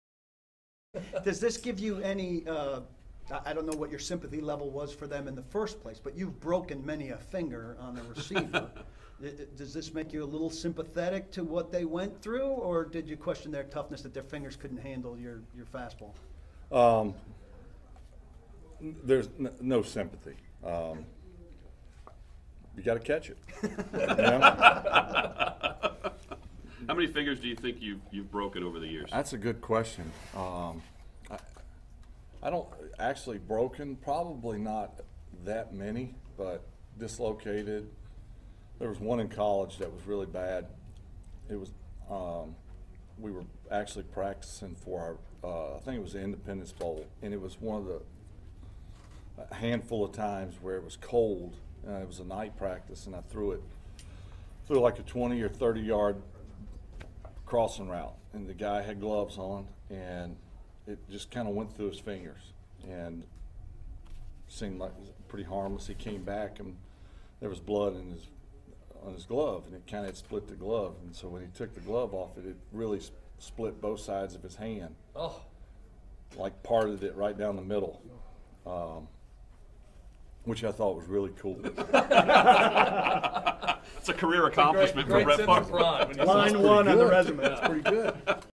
A throw from Brett Favre in college once split open his receiver's hand
Brett Favre recalls the time he threw a ball so hard, it split his receiver's fingers at the seam.